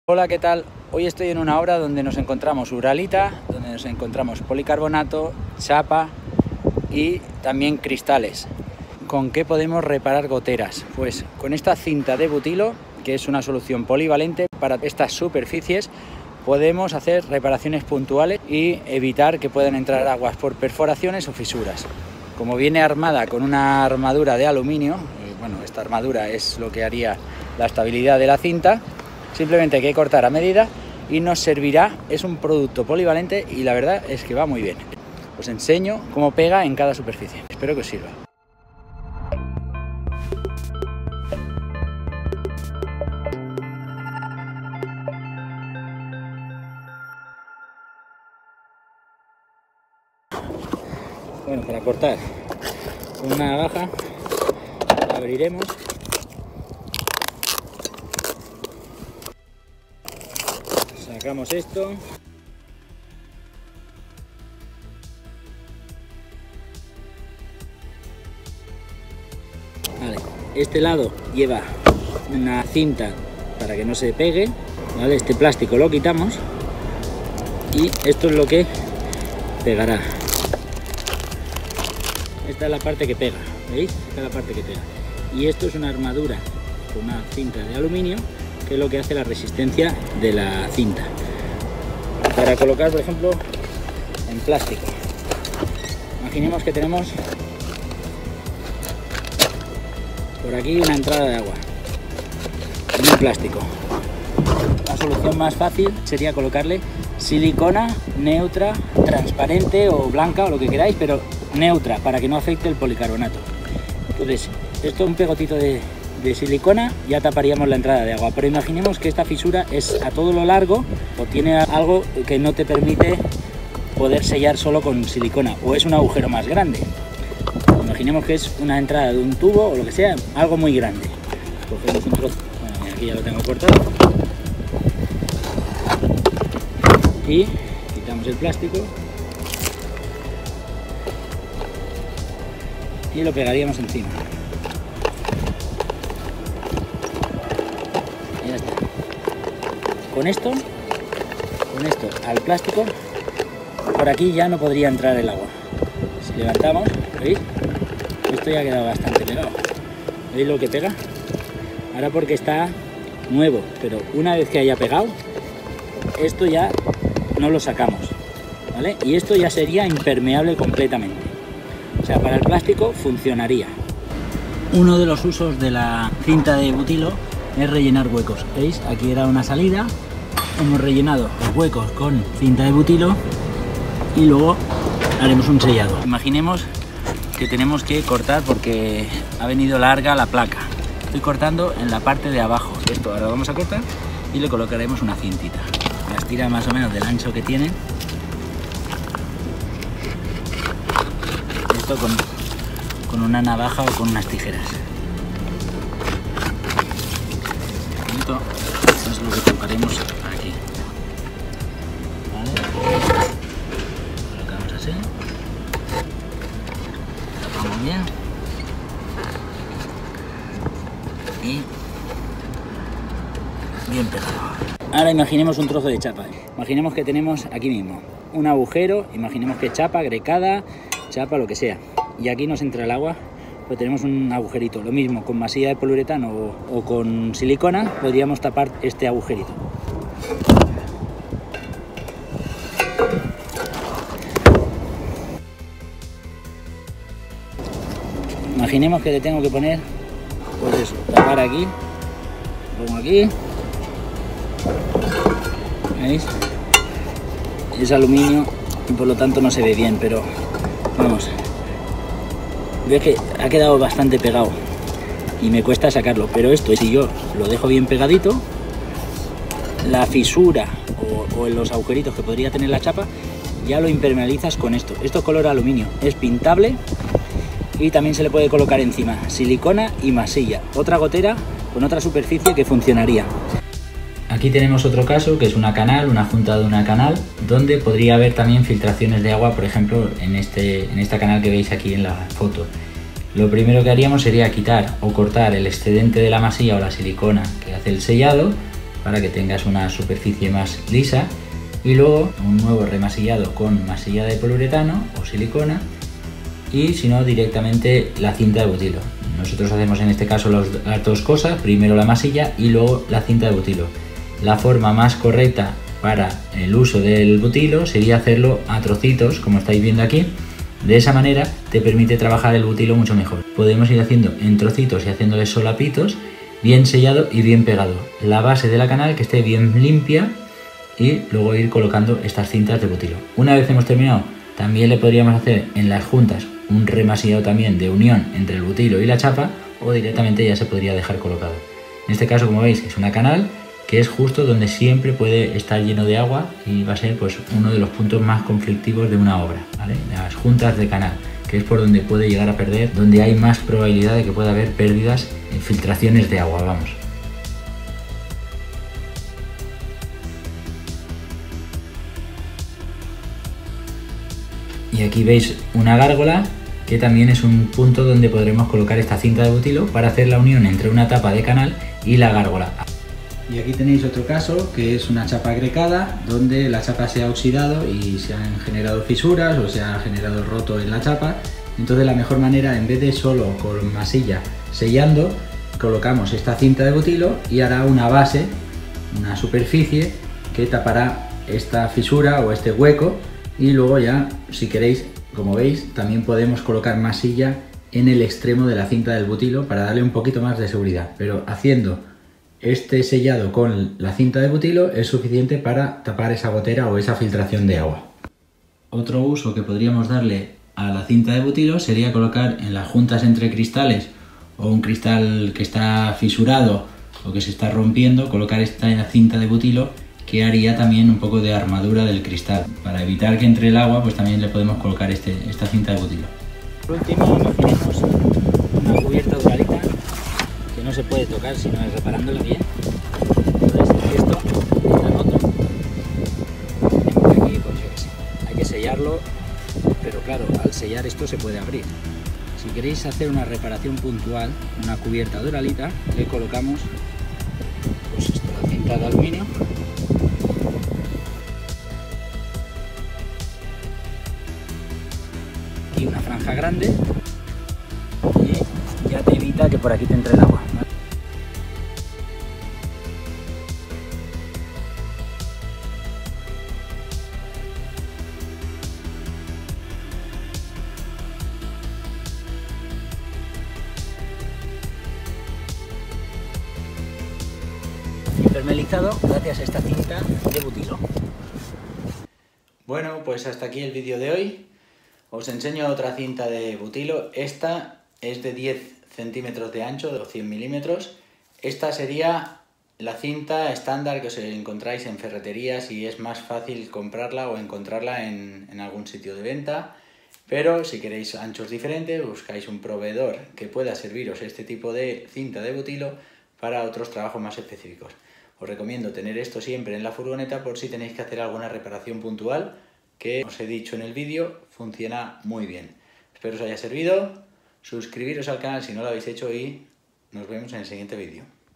👍CINTA BUTILO aplicaciones REPARACIÓN GOTERAS
Cinta de Butilo. Un producto polivalente que podemos usar para pegar y tapar huecos y sellar goteras, fisuras o filtraciones en cubiertas de chapa o plástico. Incluso en cubiertas de fibrocemento podemos usar para tapar las filtraciones.
Es un producto muy versátil que podemos usar en diferentes situaciones y para diferentes usos, en este video veréis diferentes utilidades que le damos.
No es un producto difícil de encontrar y que podremos usar en más de una ocasión. Si os dedicáis a reparar goteras aquí tenéis un aliado para vuestro trabajo. 😉
Algunos de los usos que le damos son:
- En translúcidos, plásticos de policarbonato, para el cierre de huecos o fisuras.
- En chapas grecadas para la impermeabilización de fisuras huecos o encuentros.
- En placas de fibrocemento para la reparación puntual de fisuras o huecos en cubierta.
- Y en cristales y otros materiales para tapar fisuras o goteras que puedan haber, ya que fija muy bien al pegarse.